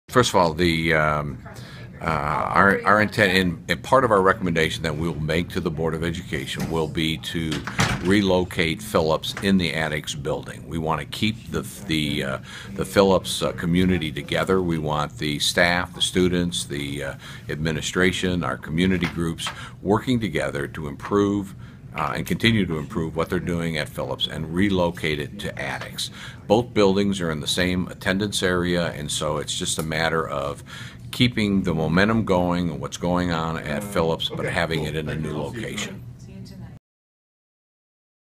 Master Plan For Kansas City Public Schools
KCPS officials plan to move students, teachers and staff from Phillips Elementary to Attucks school building